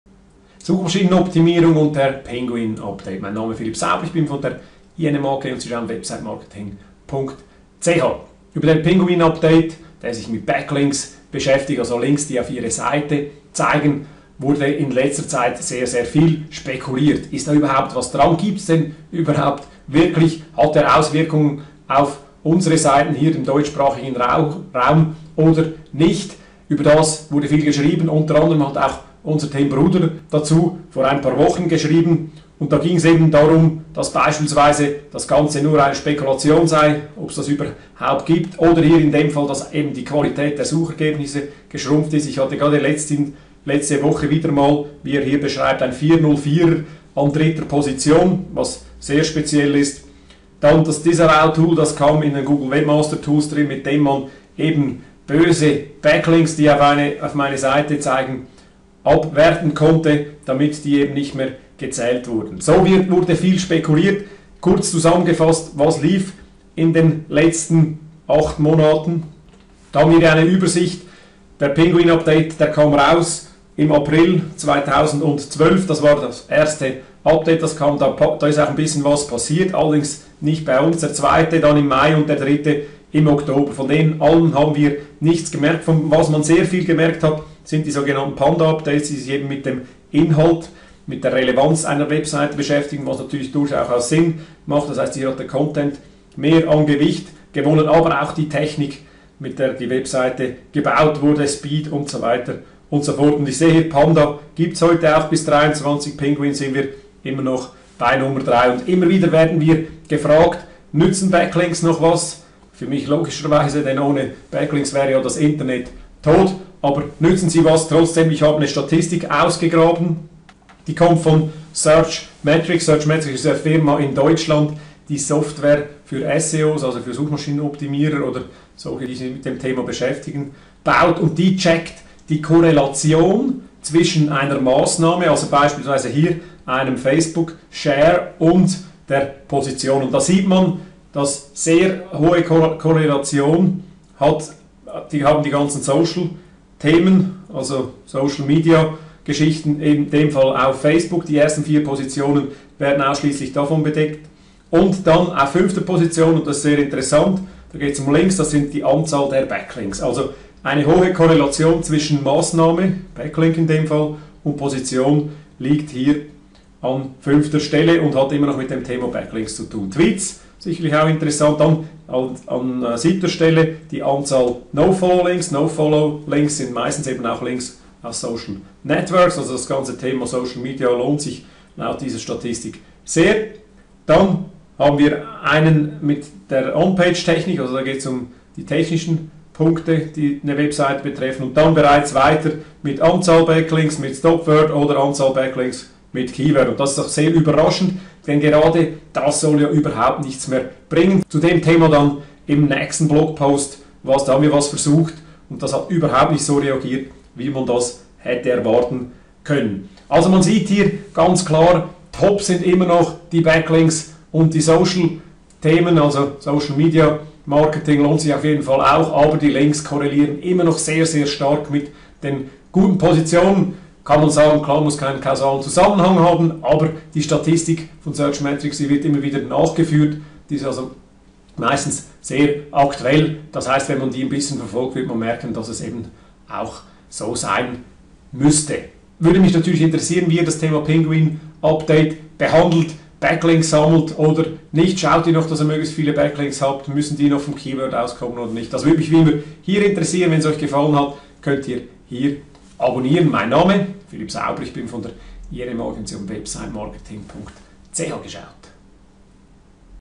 SEO und die Geschichte des Pinguin Updates hinsichtlich Backlinks
- Google schaltet immer wieder neuen Updates in ihrem Algorithmus, so auch das Pinguin Update. Pinguin überprüft die Backlinks, also Links welche auf eine Seite zeigen und deklariert die als "gut" oder eben Spam. In den letzten Monaten wurde viel über den Pinguin unter den SEO diskutiert. Einige zweifeln sogar deren Existenz an oder glauben, dass Google hier was anderes verfolgt als nur schlechte Links.
Für uns Suchmaschinenoptimierer jedenfalls ein sehr spannendes Thema, welches nicht zur Ruhe kommt. Bin gespannt wie's weiter geht.
Mein Video zeigt kurz die Historie des Google Pinguin Updates auf und beleuchtet die Wichtigkeit von Backlinks.

Auf Feedback und Verbesserungsvorschläge freue ich mich wie immer!

Danke und schöne Grüsse
Philipp Sauber
INM AG